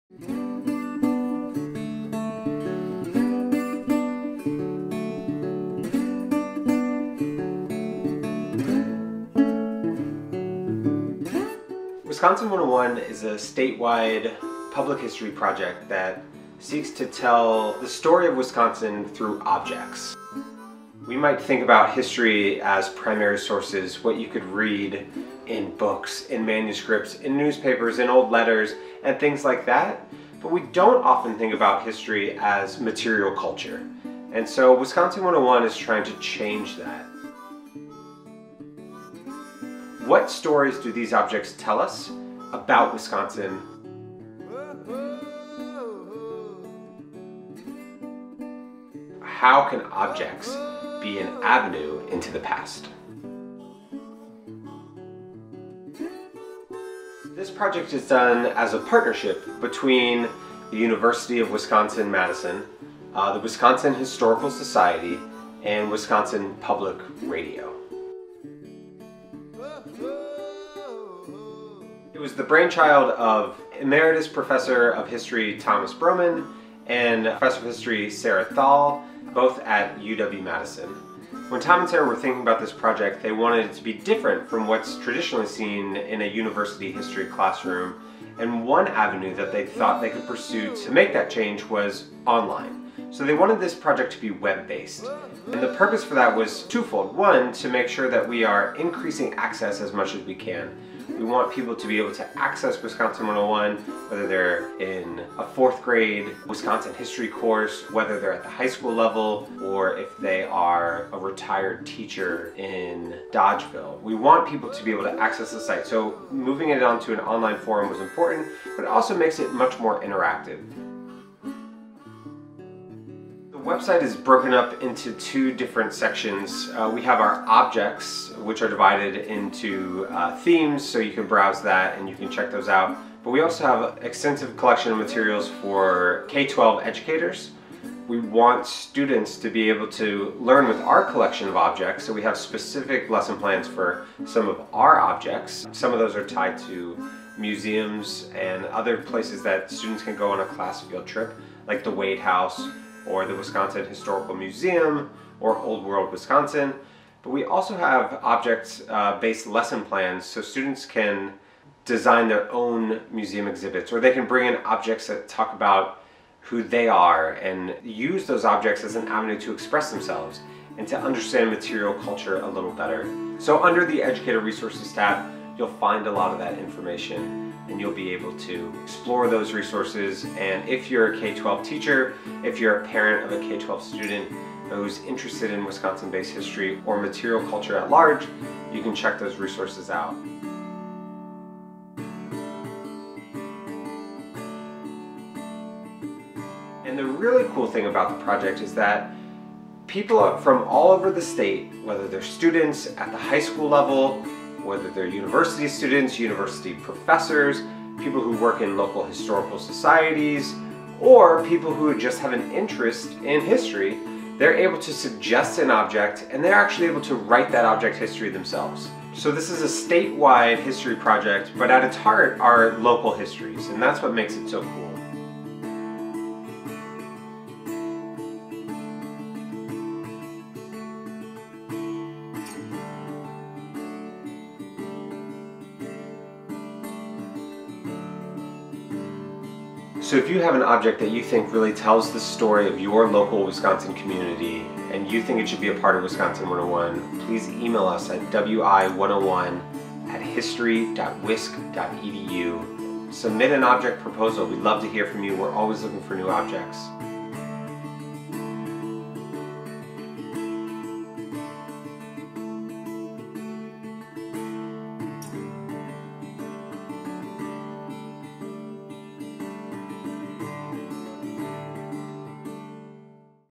Wisconsin 101: About the Project
This video explores the Wisconsin 101 project, examining both it's origins and the current iteration of the website. Learn about how to participate in this digital public history project, whether you're a student, teacher, or just a history buff. After watching the video, explore the site for yourself